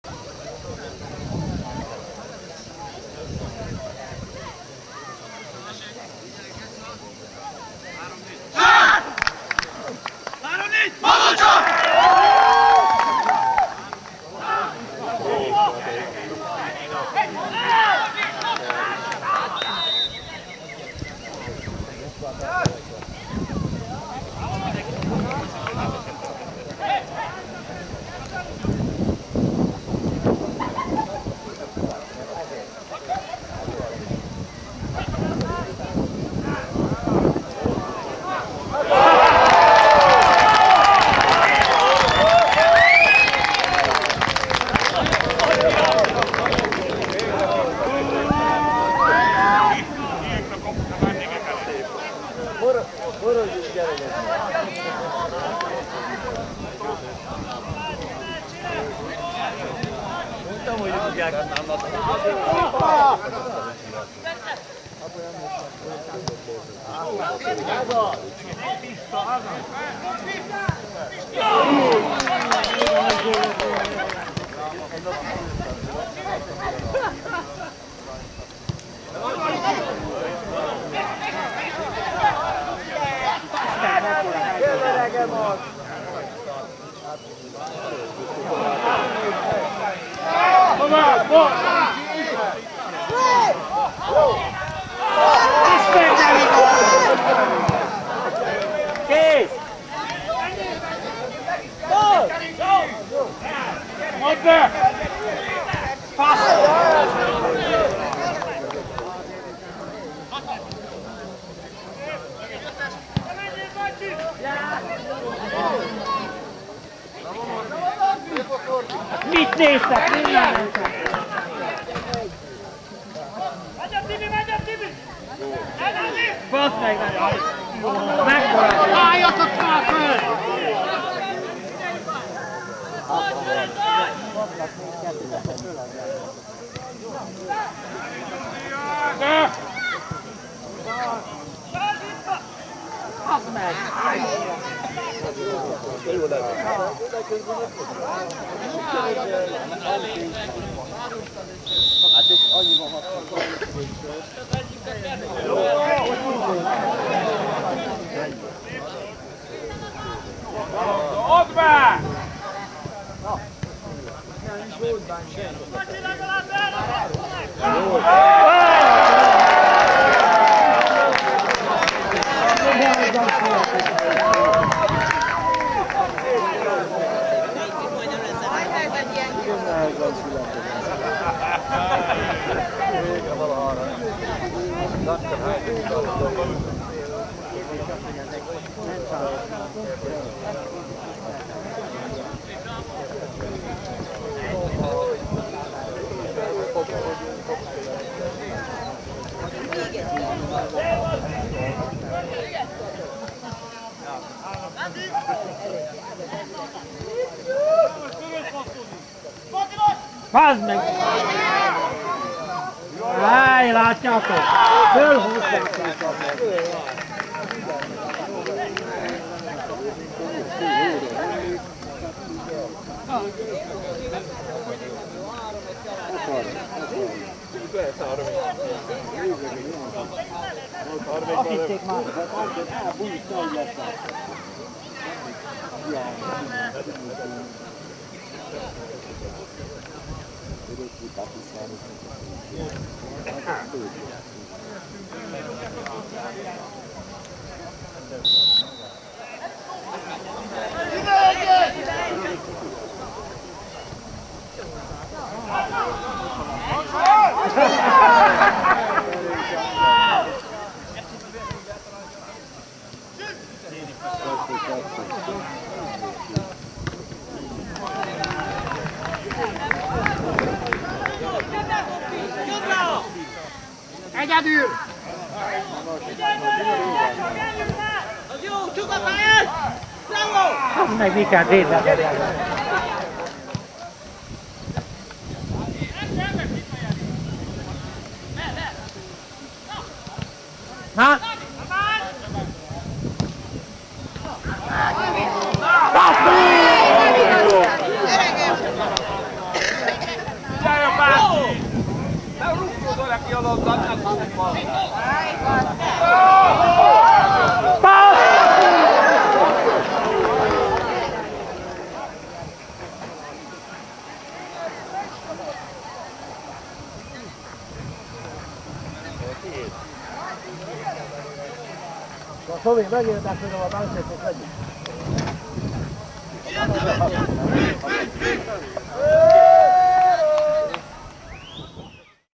Babócsa-Somogyvár foci
Babócsa-Somogyvár bajnoki labdarúgó mérkőzés összefoglalója.
Megye I.osztály - 26.forduló (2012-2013 szezon).
Babócsa 2013. május 12.